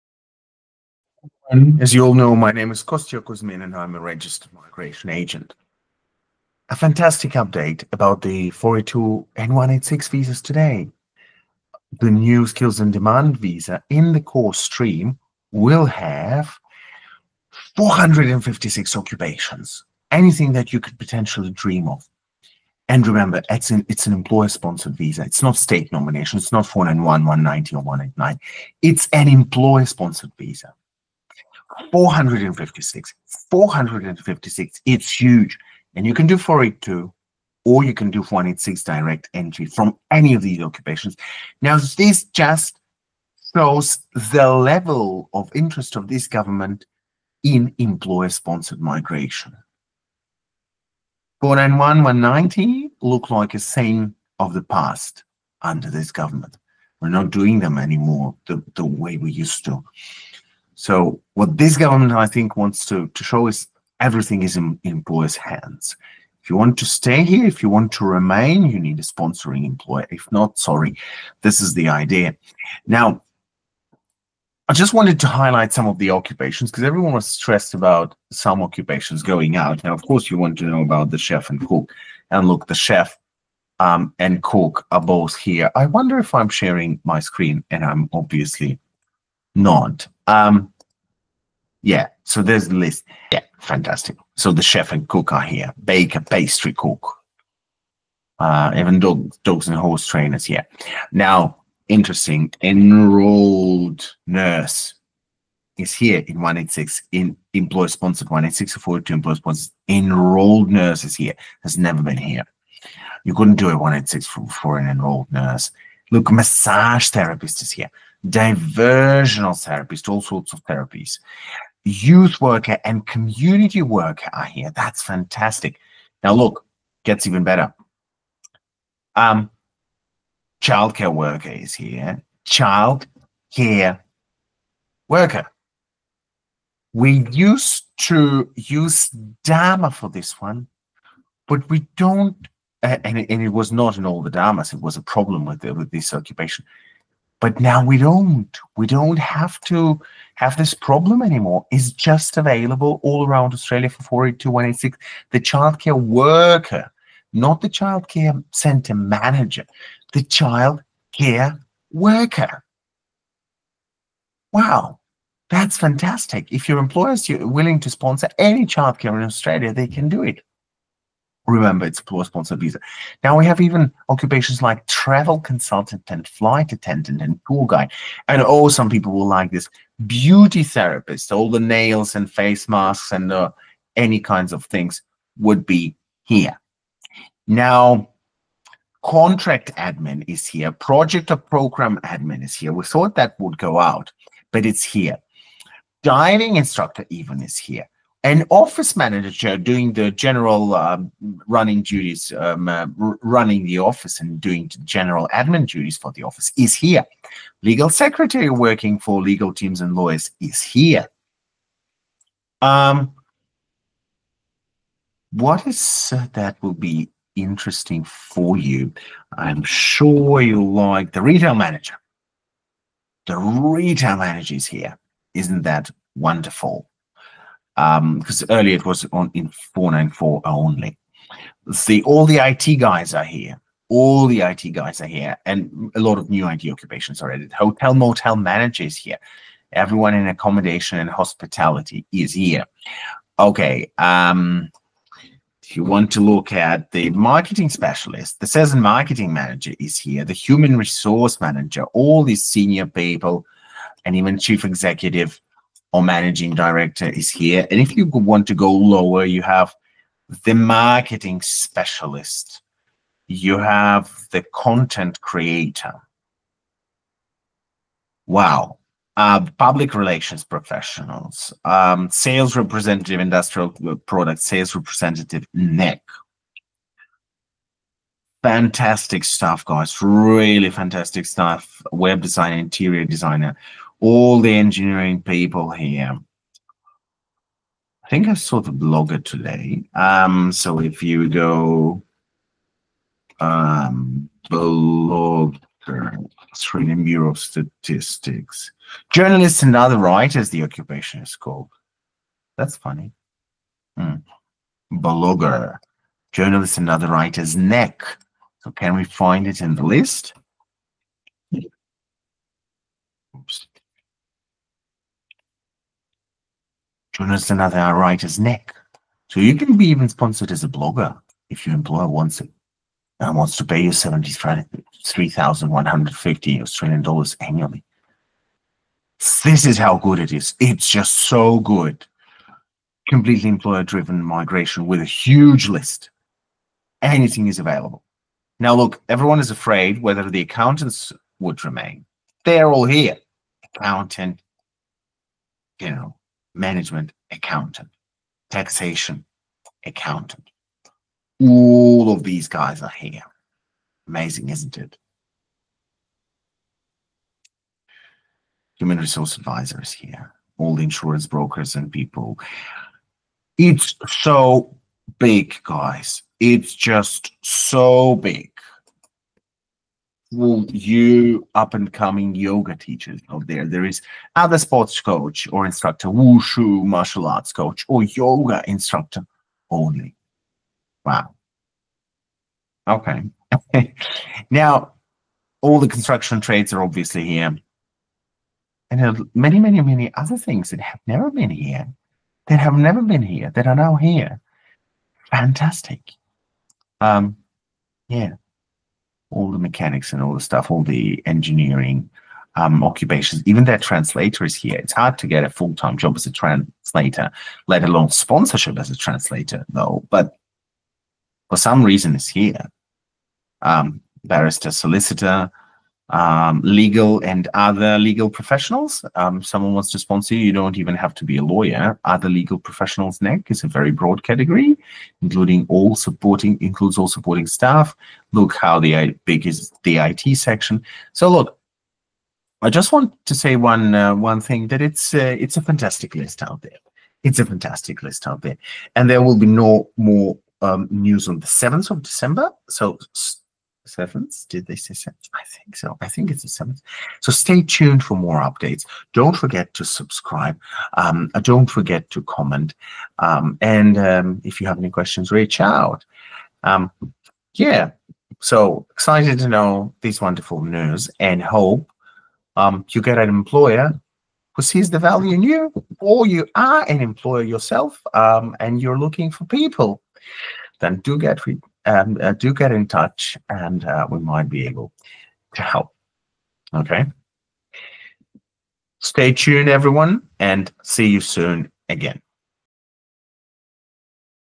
BREAKING: New occupation lists for 482 and 186 visas!
There is exciting news! Australia has updated the CSOL (Core Skills Occupations List) and it has 456 occupations!

In this video, I give an overview:
- which work visas are worth considering (482, 186 visas and others)
- a new list of occupations: extensive options for specialists in different fields that have never been available.

📍 I am a registered migration agent MARN 2117291 (Australia).